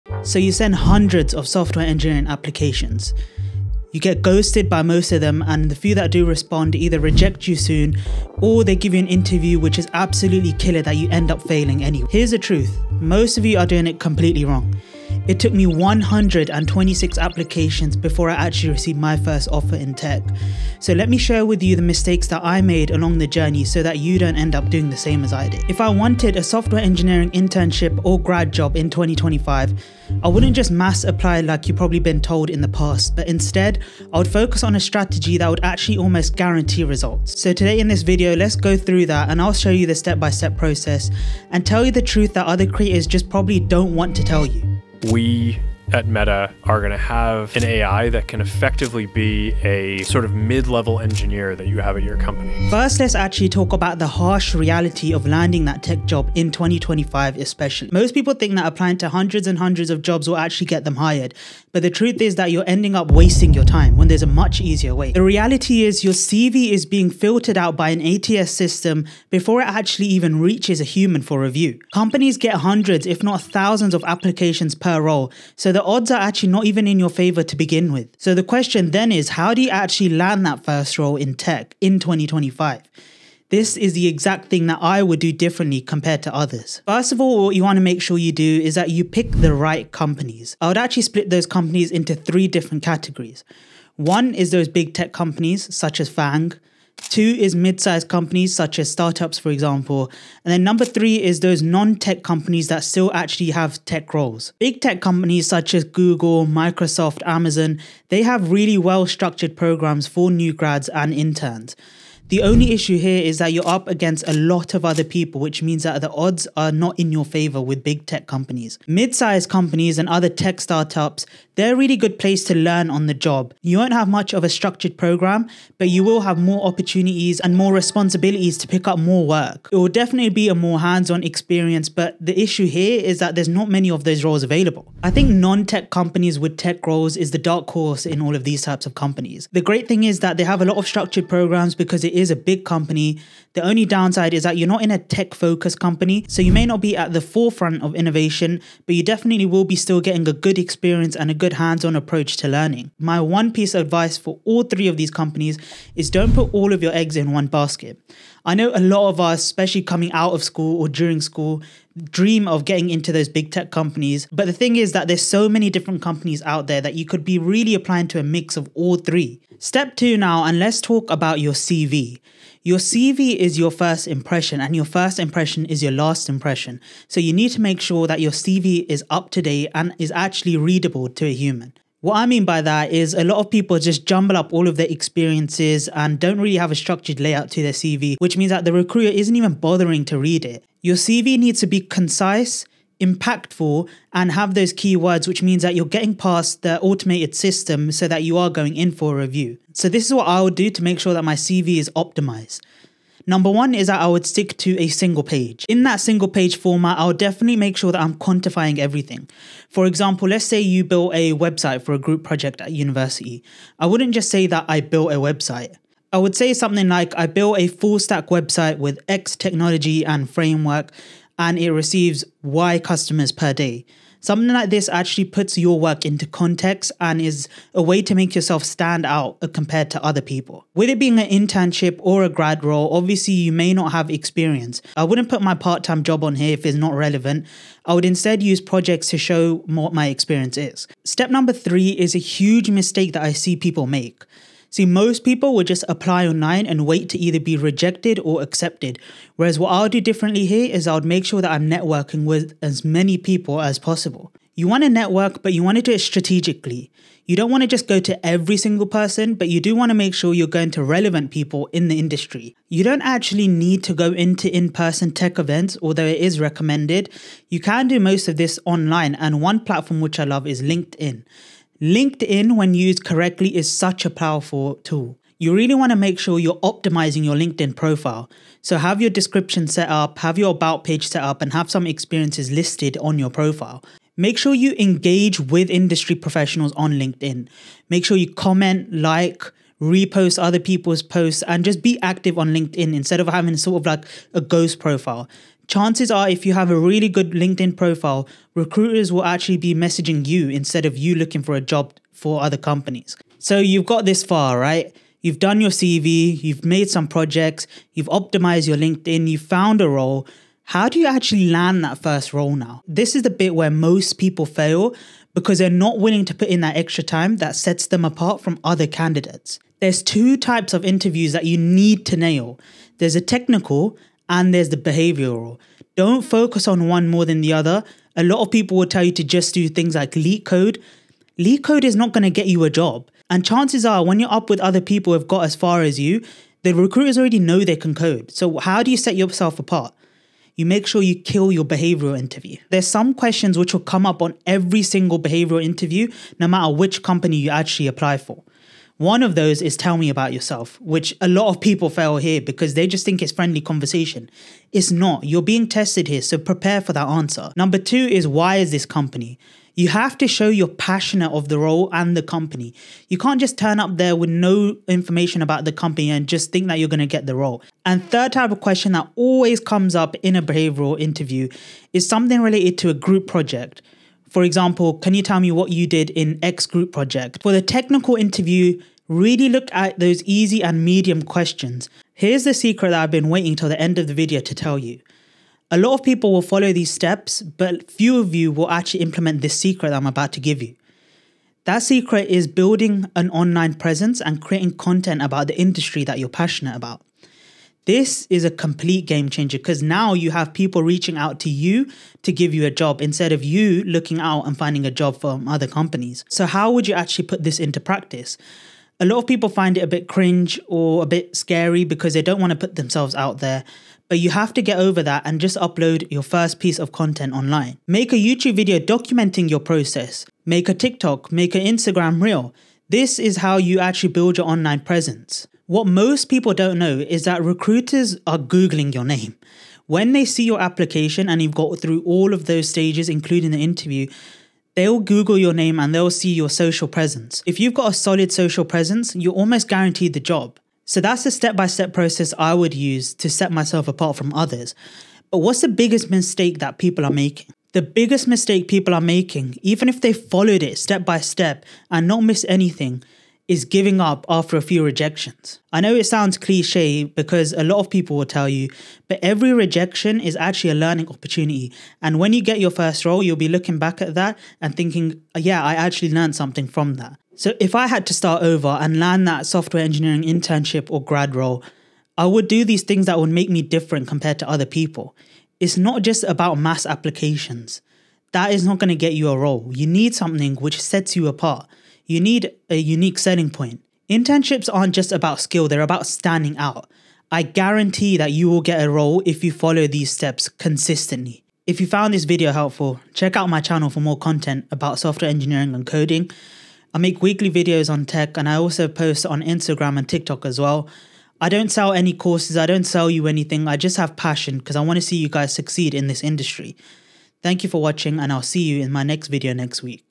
The FASTEST Way to Become a Software Engineer in 2025 | NO DEGREE | NO BOOTCAMP (Step By Step Guide)
Hey everyone! Let me show you my detailed step by step plan to become a Software Engineer in 2025.

Timestamps:
00:00 - Intro
00:52 - Harsh Reality
01:26 - Picking the right company
02:49 - CV/Resume
04:01 - HUGE mistake people make
05:03 - The FINAL step
06:22 - Taking action
07:20 - GIVING UP
07:47 - Final thoughts